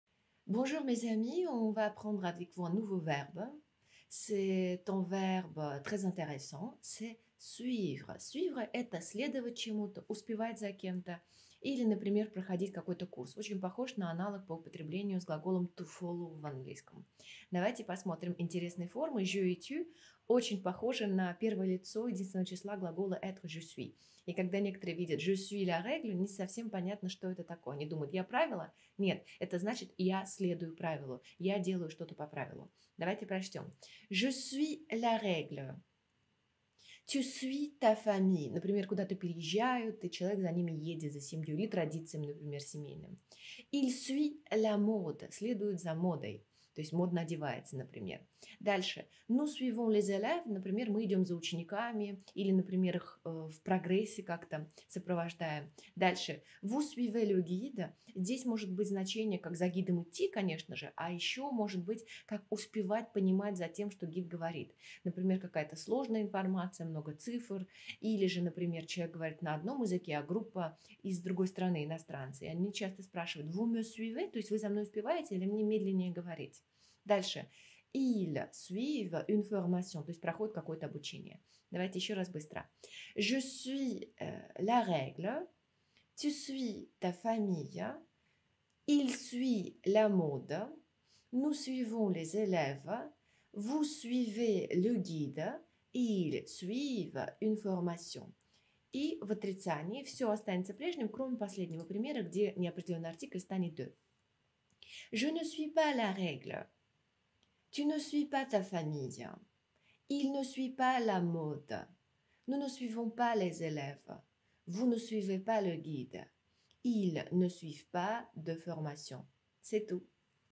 Cпряжение в présent №53: suivre
Сегодня знакомим вас с новым глаголом: suivre – следовать, следить, идти за кем-то.

В этом видео мы выучим спряжение глагола в настоящем времени, а также несколько примеров употребления.